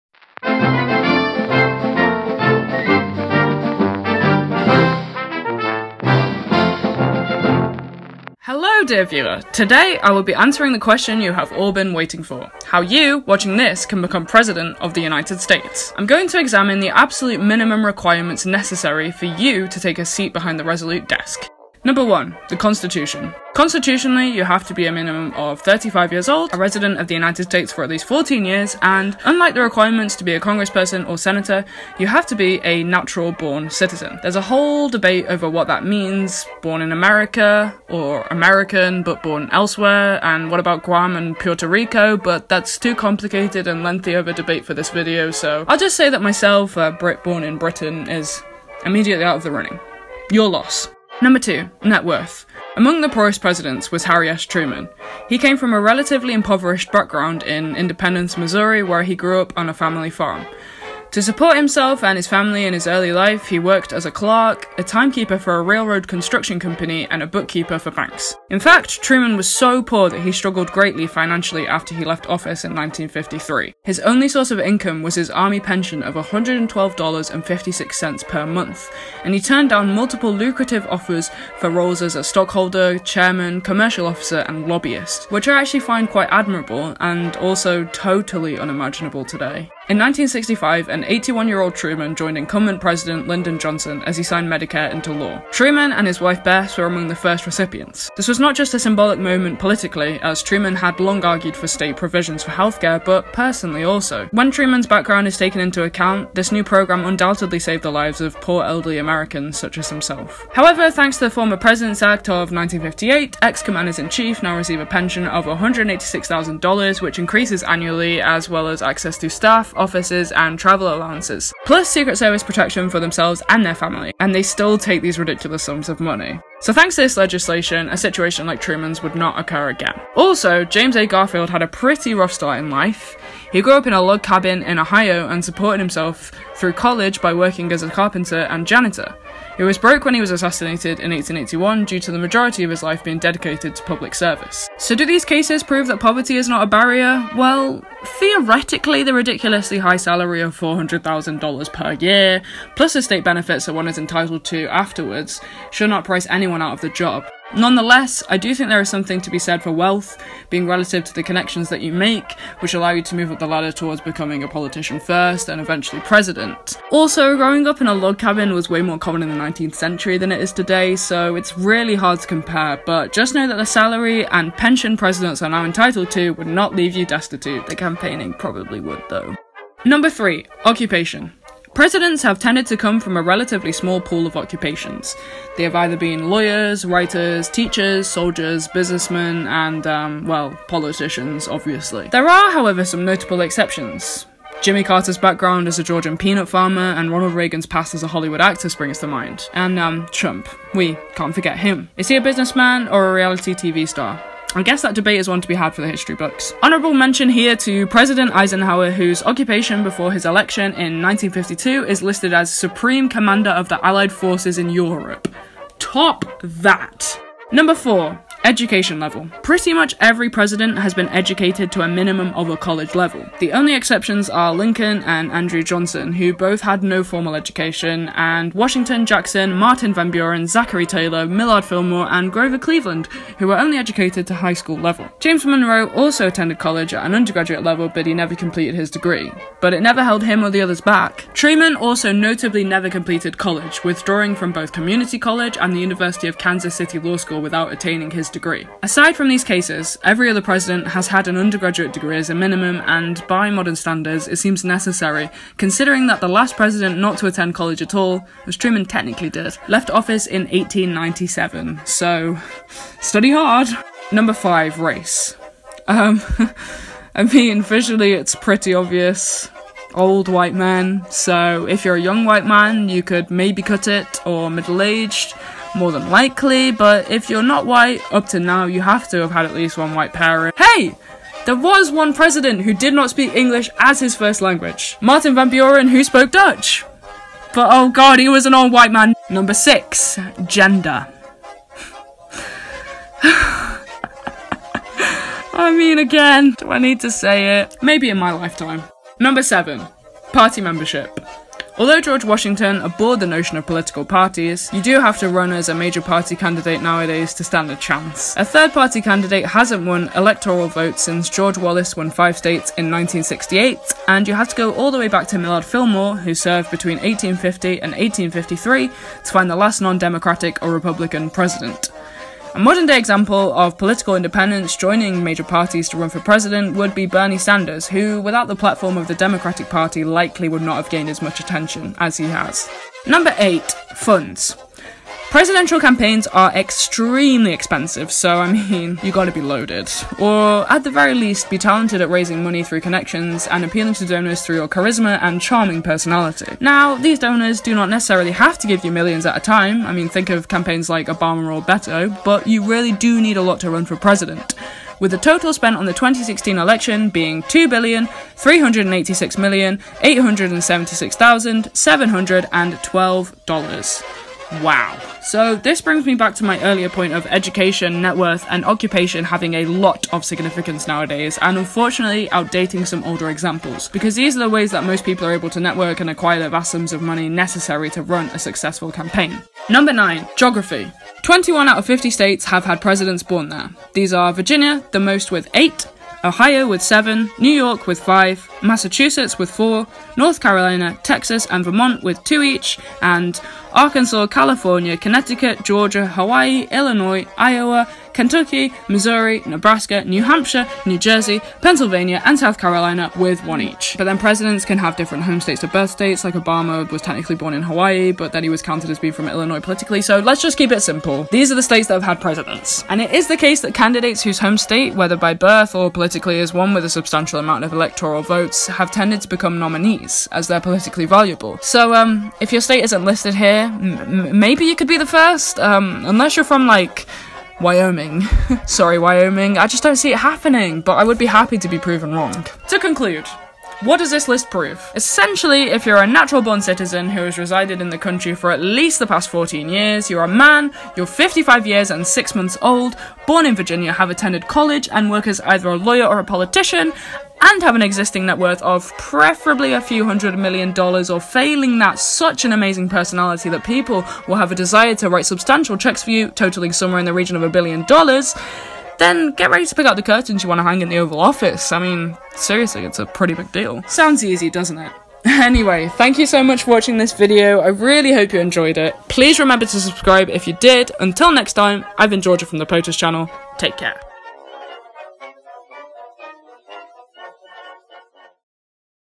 How To Be President
Do you think you could become President with these criteria in mind?

4. James A. Garfield's background

(Did anyone notice I was gone for two years? 👀)